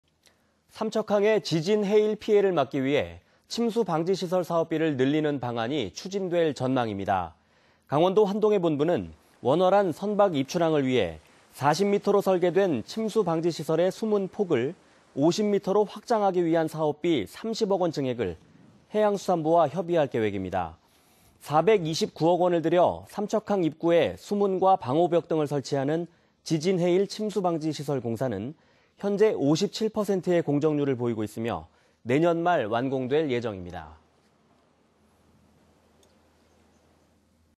[단신]삼척항 지진 해일 방지 사업비 증액 검토190403
삼척항의 지진 해일 피해를 막기 위한
침수 방지 시설 사업비를 늘리는 방안이
검토되고 있습니다.
           ◀ＥＮＤ▶

강원도환동해본부는 원활한 선박 입출항을 위해 40m로 설계된 침수 방지 시설의 수문 폭을
50m로 확장하기 위한 사업비 30억원 증액을
해양수산부와 협의할 계획입니다.

  429억 원이 투입해 삼척항 입구에
수문과 방호벽 등을 설치하는 지진 해일 침수 방지 시설 공사는 현재 57%의 공정률을 보이고 있으며, 내년 말 완공될 예정입니다.